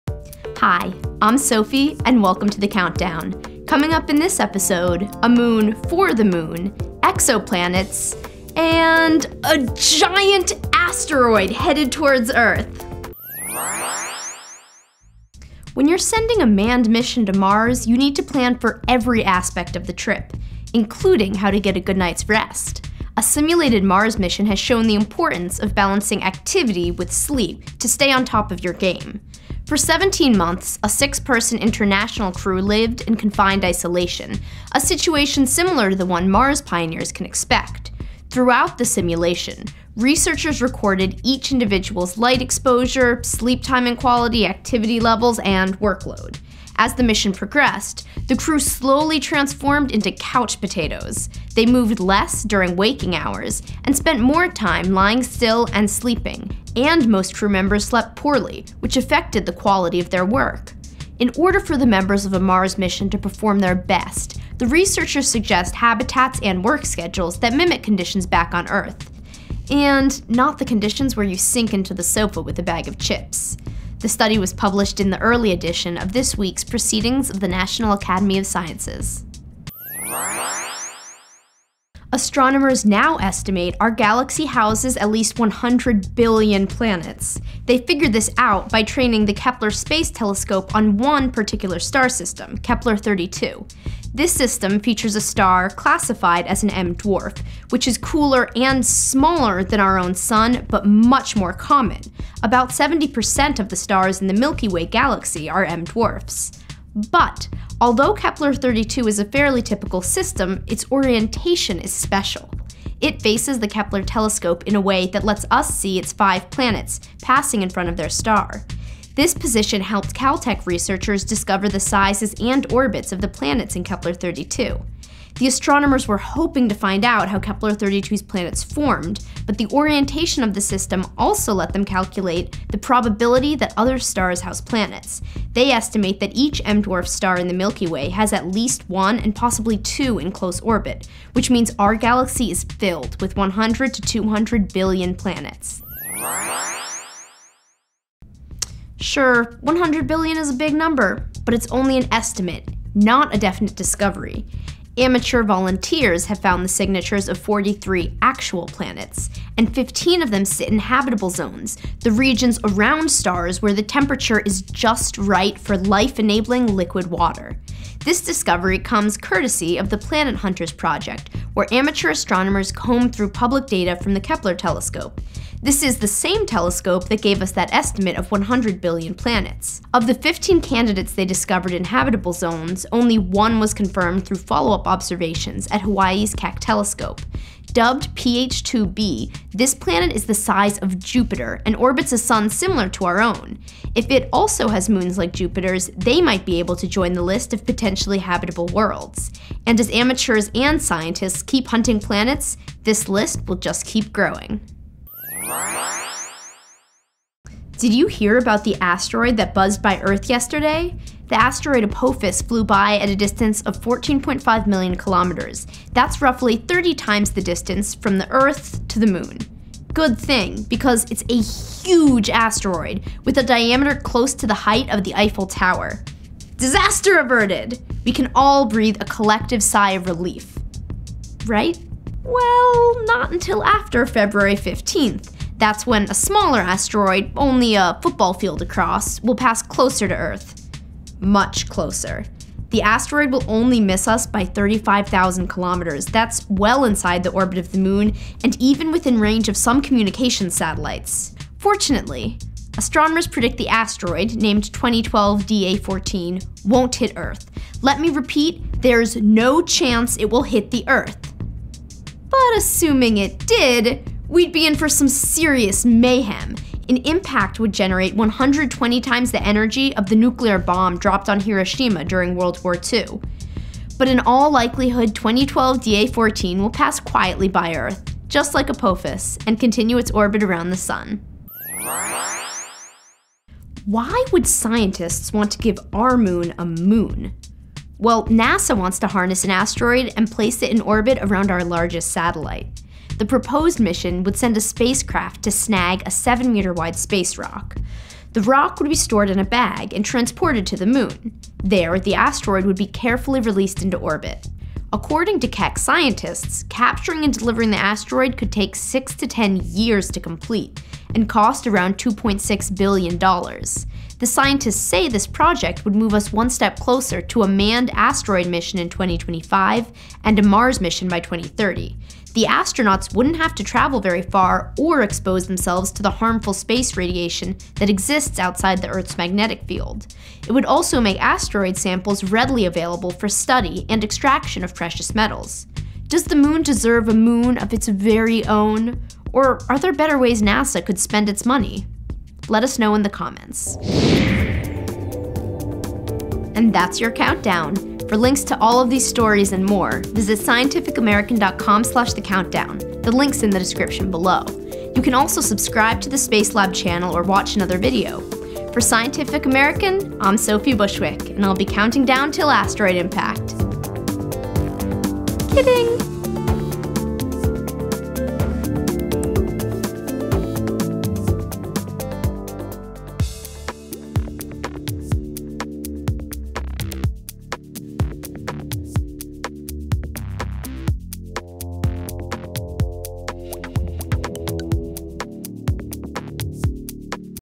Couch Potatoes on Mars! - The Countdown #13
THE TOP 5 are listed BELOW!!

THE TOP 5:
Couch Potatoes on Mars
100 Billion Exoplanets
Amateur Discovery
Asteroid Flyby
A Moon for the Moon